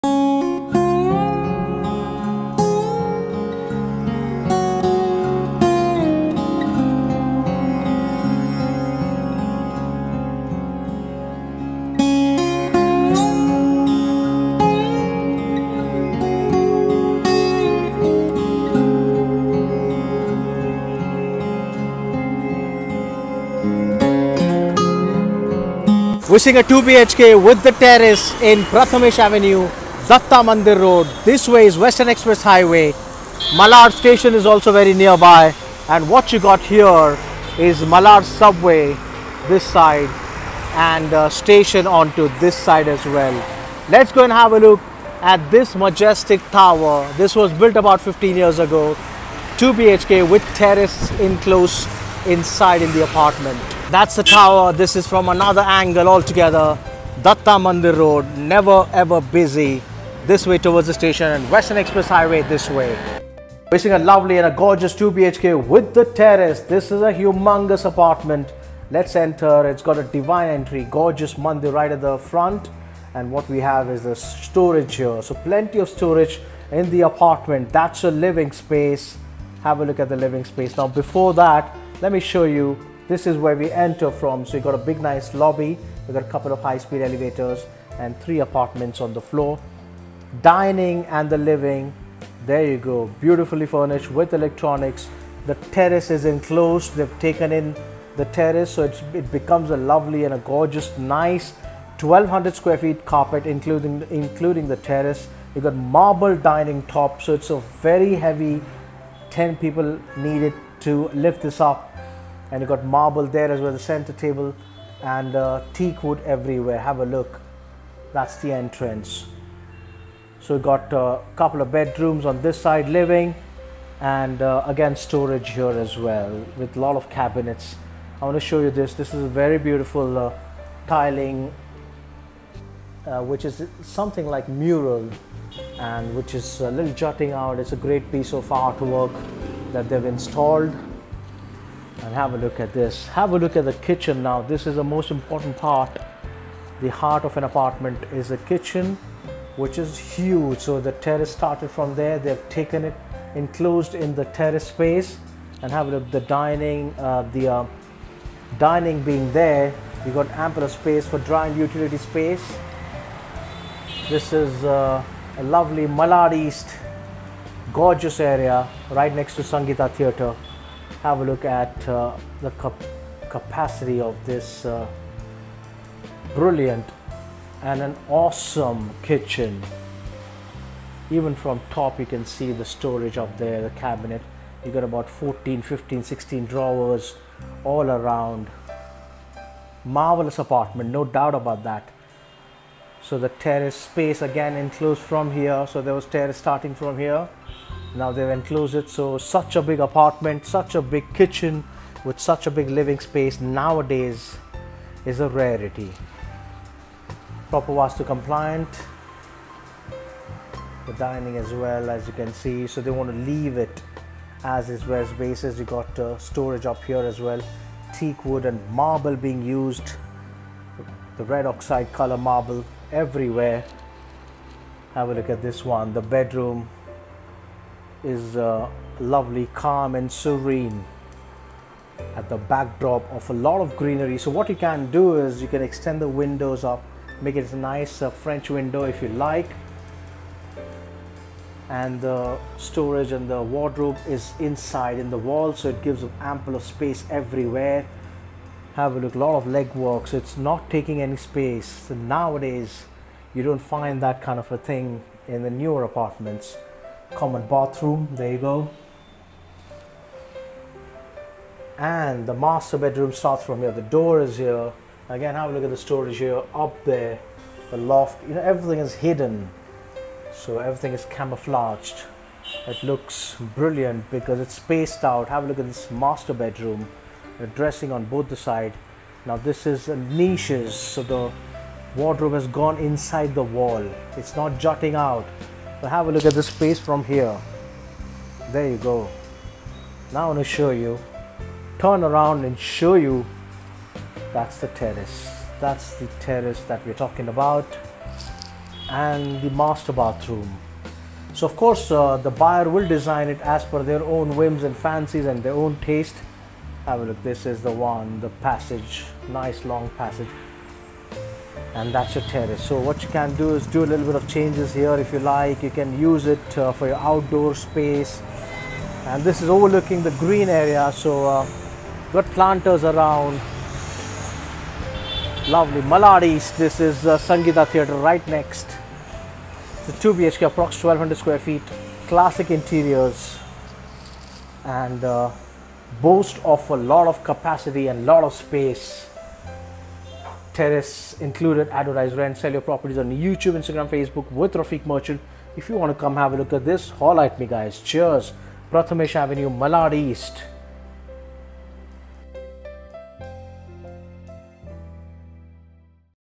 2.20 Crore, 2bhk with Terrace, Prathamesh Avenue, Datta Mandir Road, Malad East
2 BHK WITH TERRACE FOR SALE

Hall - 1
Bedroom- 1
Master Bedroom - 1
Kitchen- 1
Toilet - 2
Terrace - 1

Area - 1200 sq ft carpet area,after Extensions.

Address - PRATHAMESH AVENUE ,
 Datt mandir road , malad east

Quote Price - 2.20 CRORE

✅ House with Classic Interior and modular kitchen

✅ lavish terrace with great view

✅ Ready to move with all electronics and equipments

✅ 5 min walk from nana nani park

✅ 5 min away from kurar metro station

✅ 1 min from Western Express Highway

✅ 5 min walk from malad railway station

ADVERTISE, BUY, SELL, RENT YOUR PROPERTIES ON YOUTUBE INSTAGRAM FACEBOOK WITH RAFIQUE MERCHANT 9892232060, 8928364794